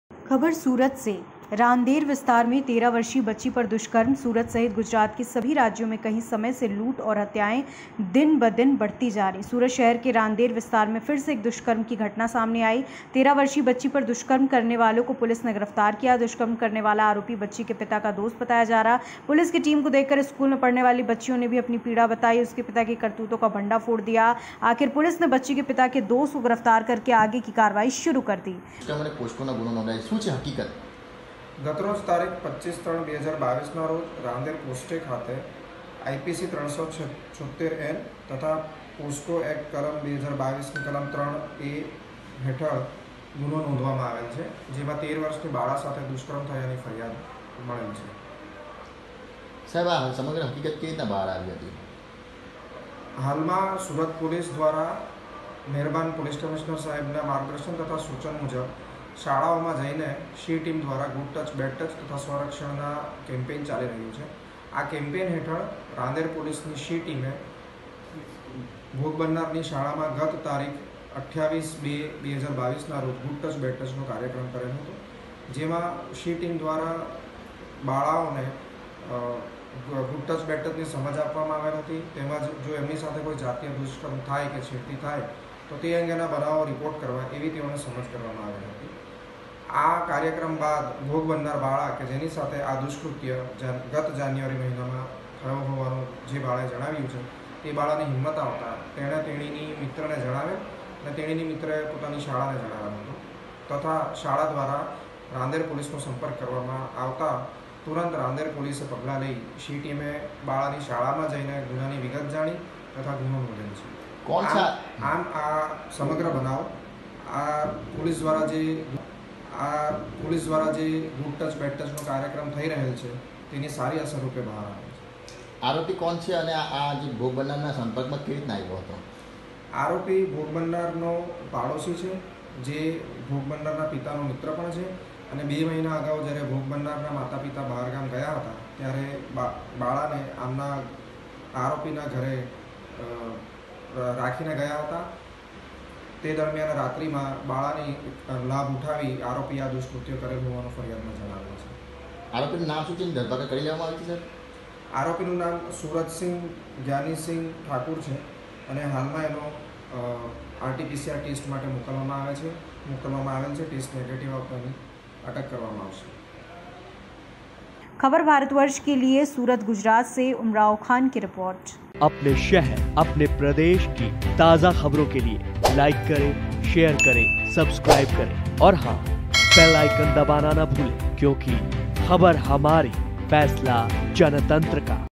सूरत (गुजरात) : रानदेर विस्तार में 13 वर्षीय बच्ची पर दुष्कर्म ,आरोपी बच्ची के पिता का दोस्त
सूरत (गुजरात) : रानदेर विस्तार में 13 वर्षीय बच्ची पर दुष्कर्म ,दुष्कर्म करने वाला आरोपी बच्ची के पिता का दोस्त